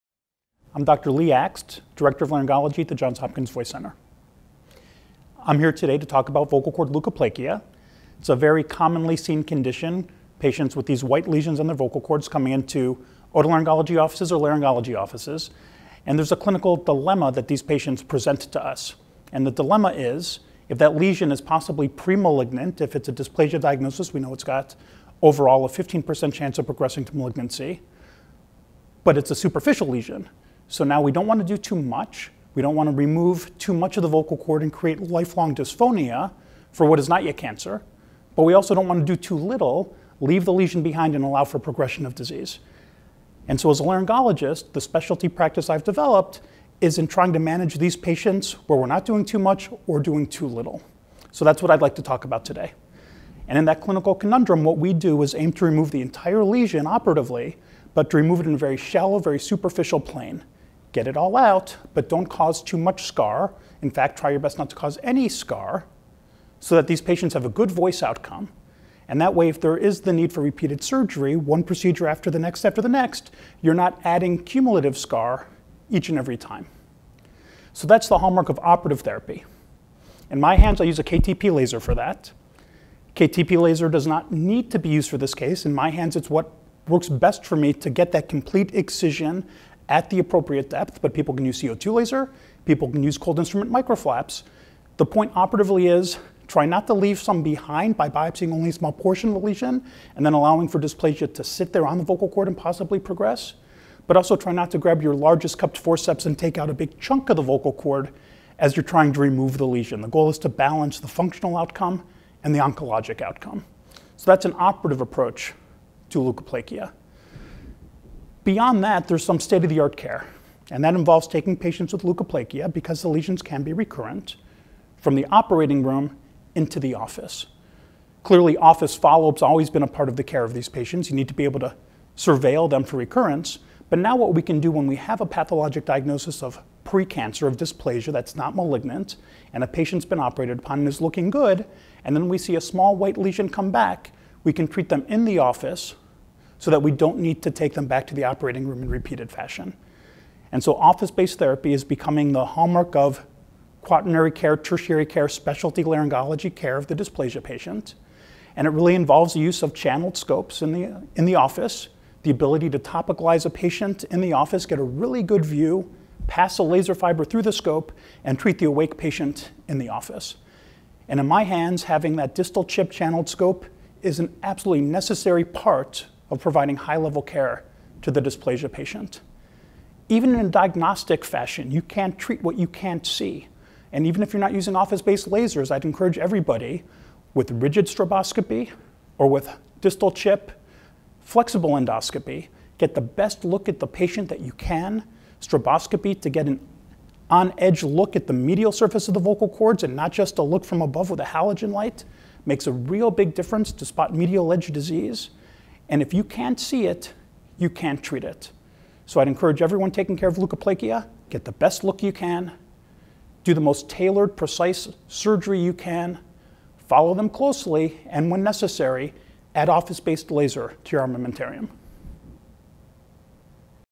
Leukoplakia treatment
Lee Akst, Director Laryngology, John Hopkins Voice Center, discusses leukoplakia treatment